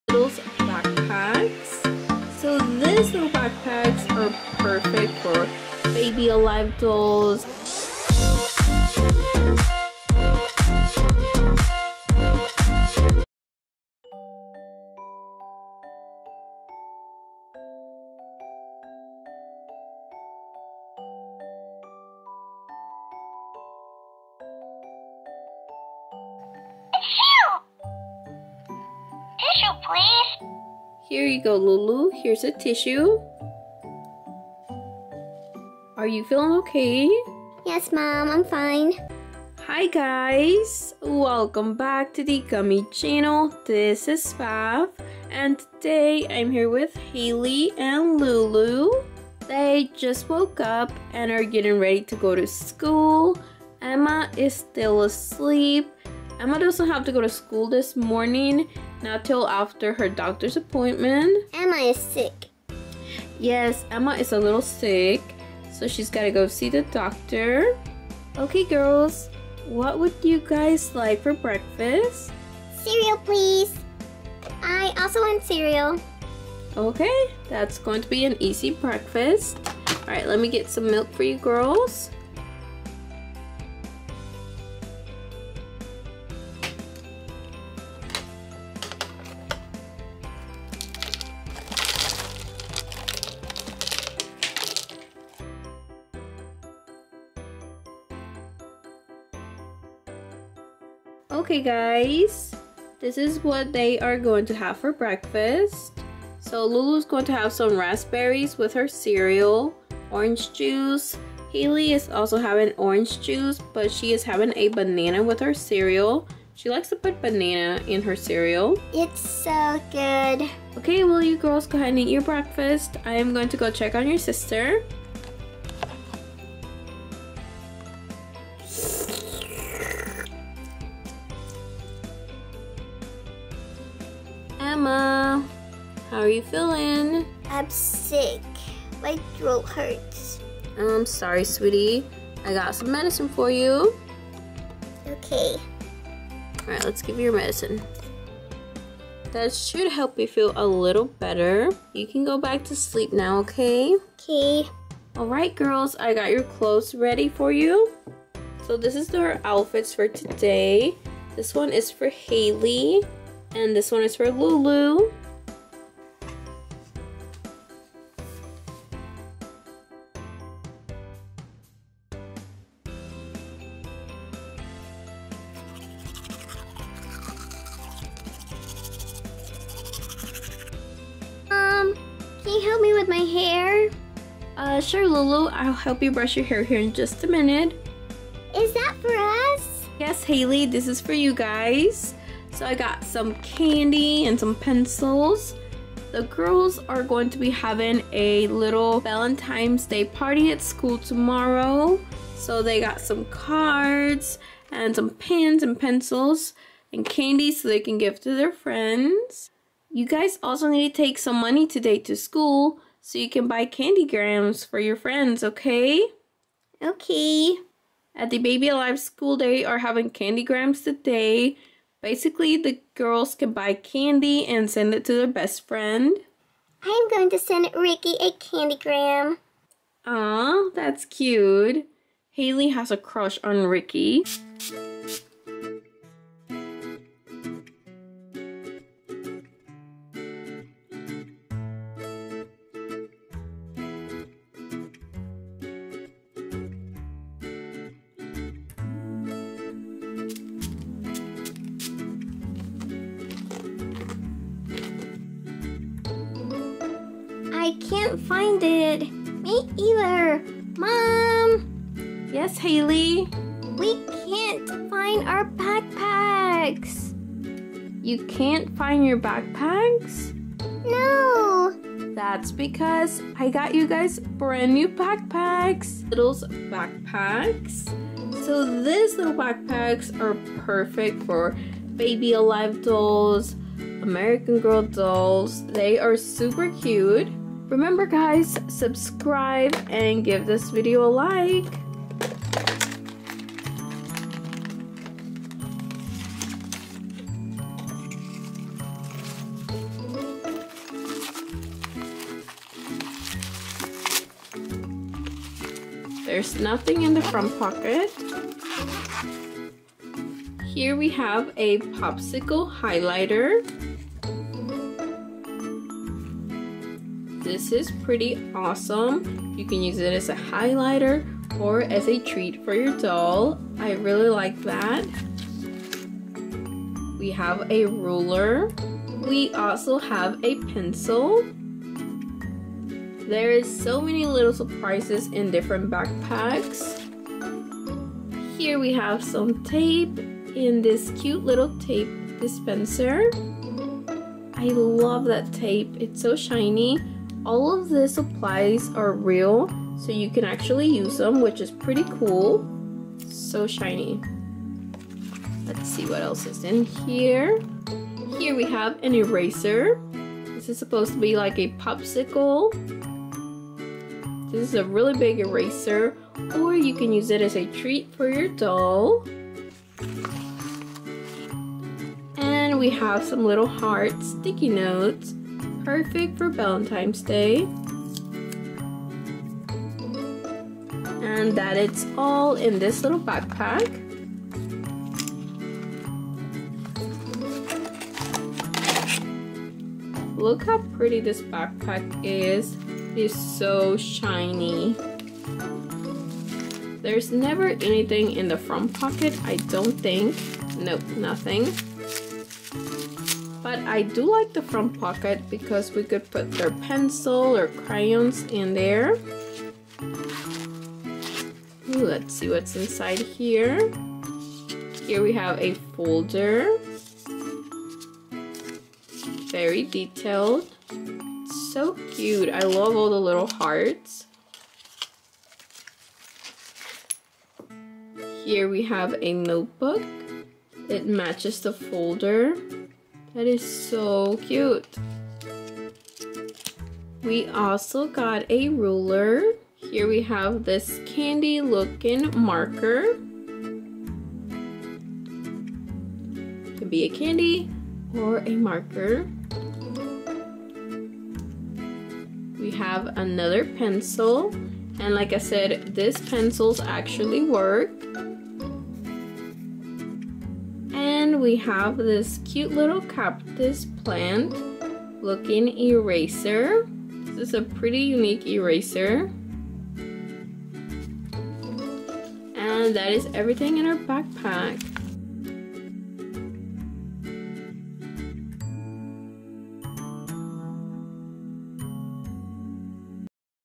Baby Alive Dolls Packing Their New Backpacks 🎒 📚
Baby Alive Dolls Packing Their New Backpacks with mini surprises